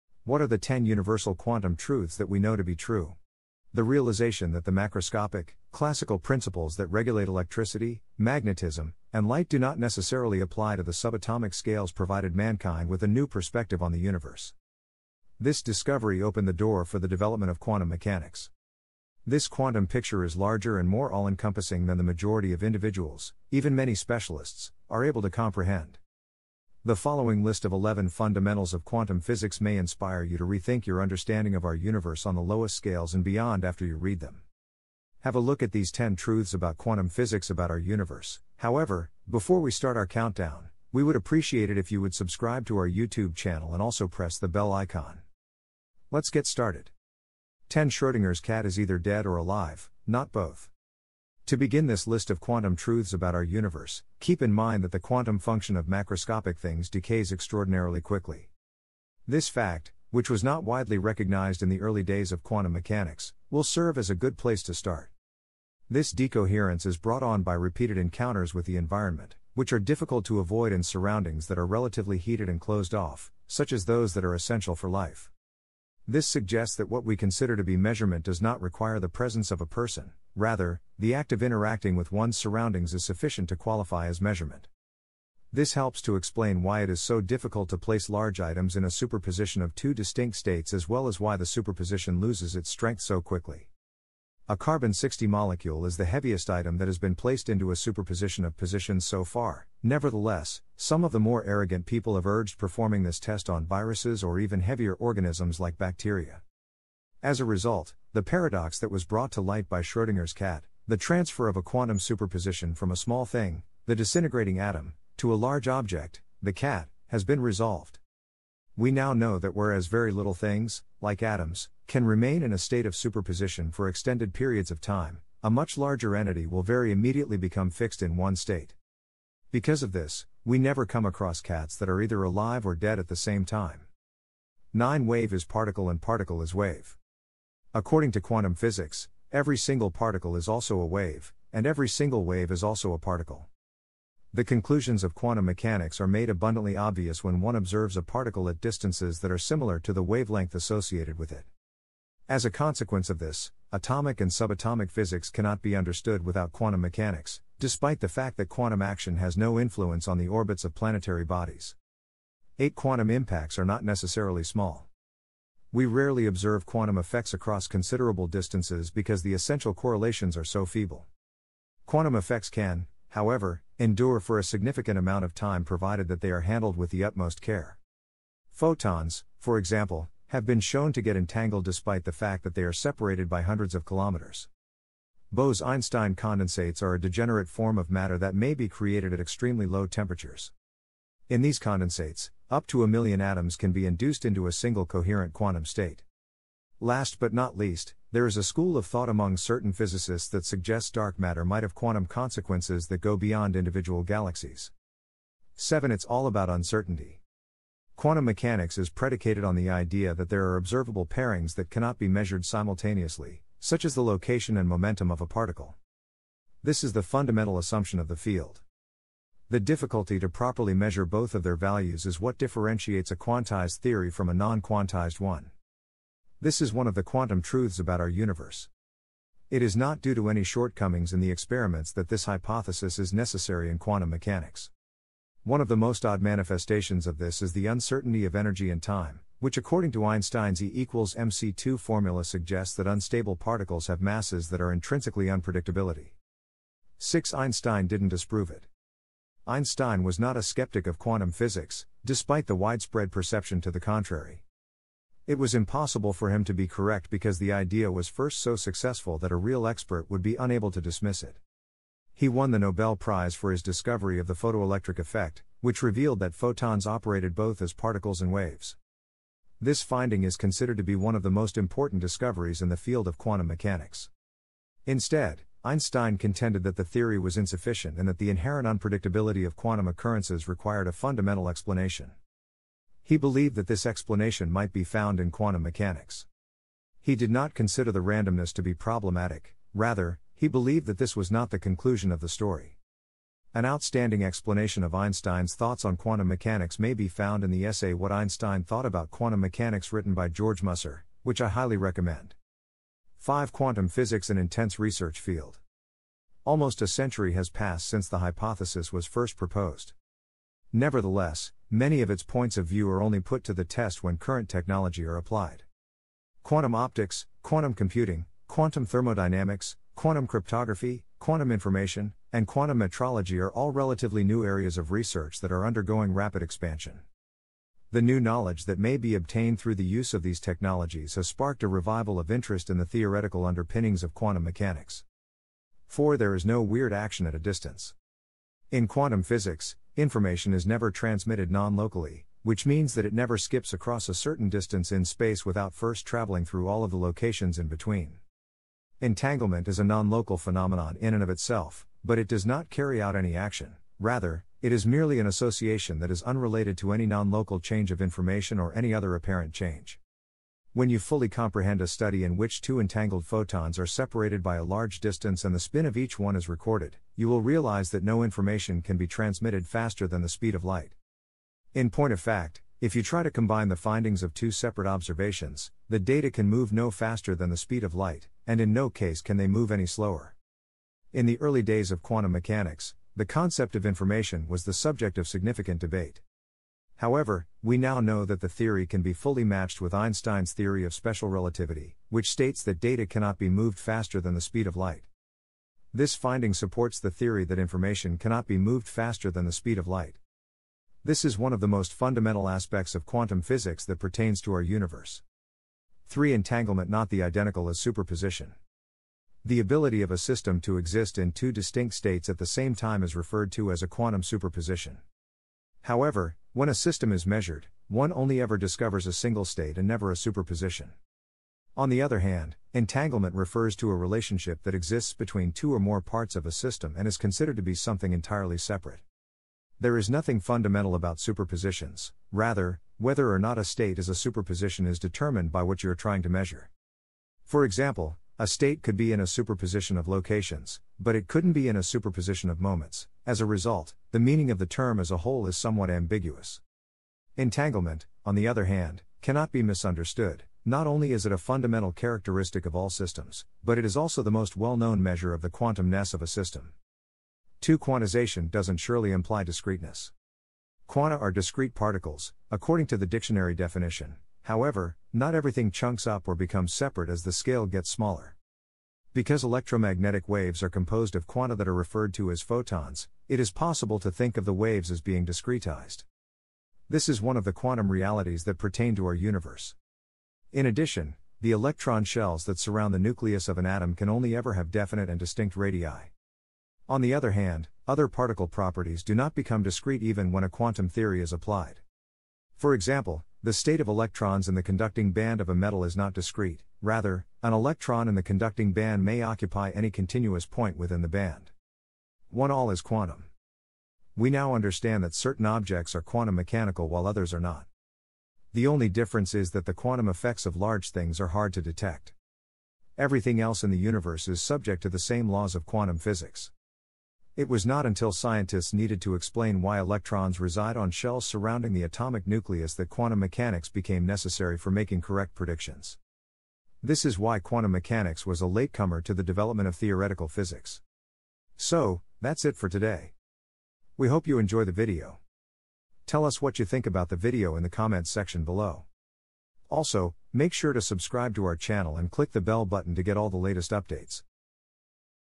10 QUANTUM TRUTHS ABOUT OUR UNIVERSE
From the moment it was discovered that the macroscopic, classical rules that dictated electricity, magnetism and light didn’t necessarily apply to the subatomic scales, an entirely new view of the Universe became open to mankind. This quantum picture is much larger and all-encompassing than most people understand, including many specialists. Here are ten basics of quantum mechanics that may make you re-examine how you picture our Universe, on the smallest scales and beyond.